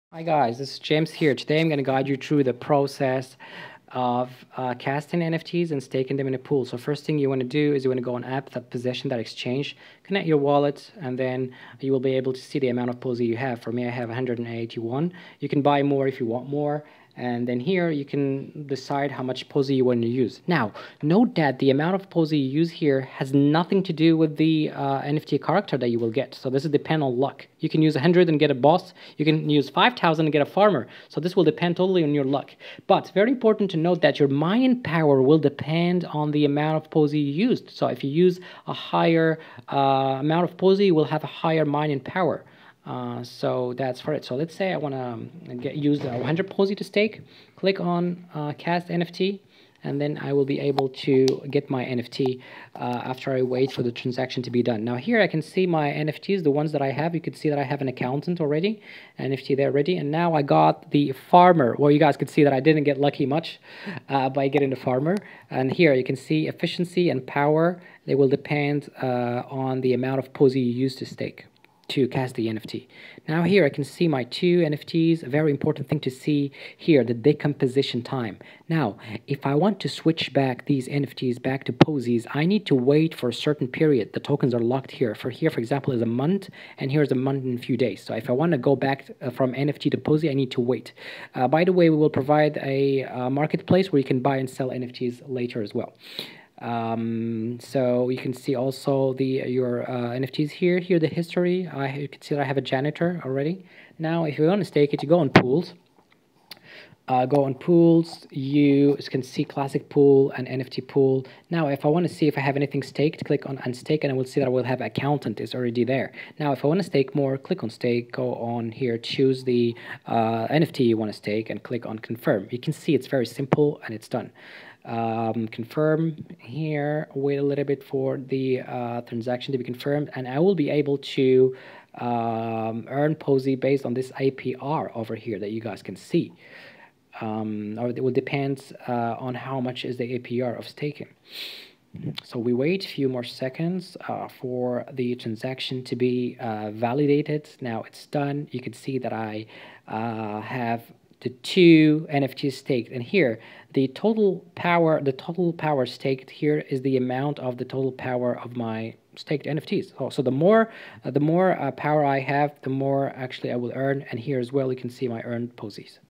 How to Cast and Stake POSI NFT By Position Exchange
1. Deposit POSI tokens in the NFT tab
2. Get rewarded with NFT cards with different rarities and characteristics (The better your NFT is, the higher your mining power will be)
3. Stake the NFT cards in the NFT Pool.
4. Earn POSI.

🚀 To swap, stake or farm POSI copy Link below paste in your Trust Wallet DAPP to connect to Position Exchange